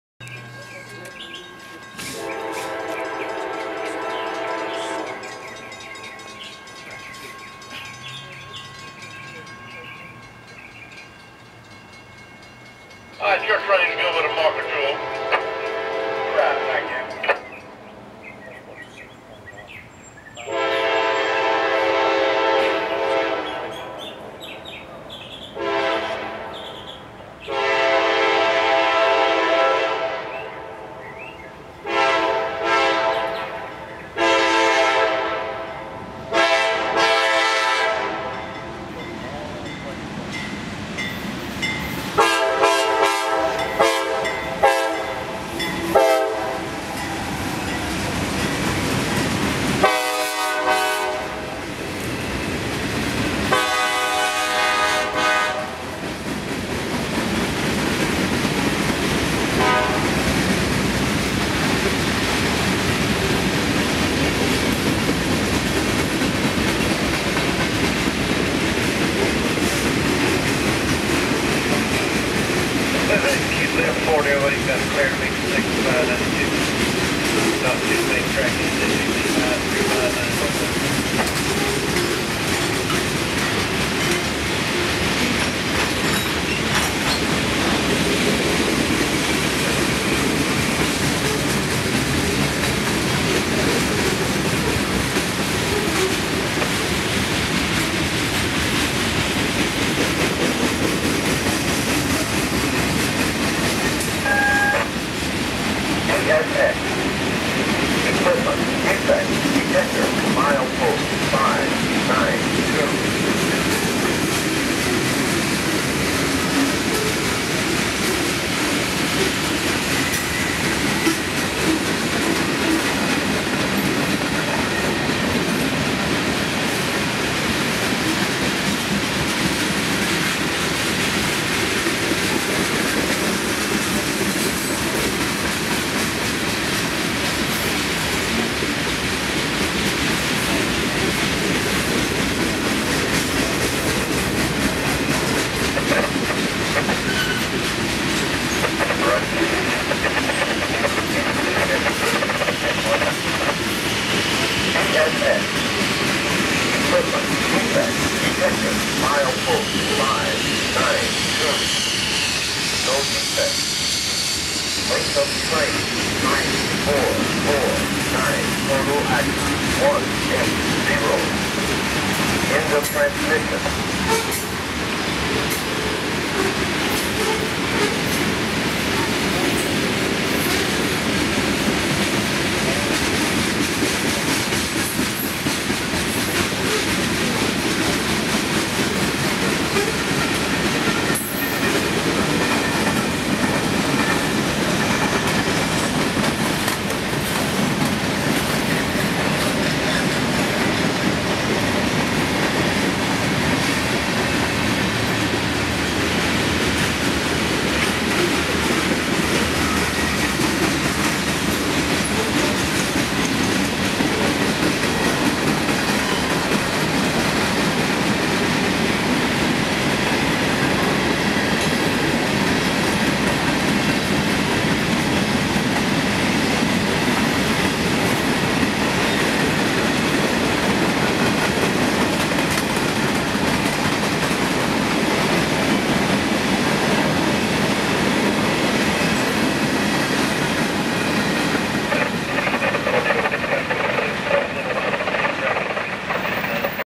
Southbound CSX Manifest Through Folkston
CSXT CW40-8 #7340 leads a mixed freight into Florida from Waycross in October. Listen to to the hissing sound around 2:42 Hopefully it was just an airhose and not a leaking chemical.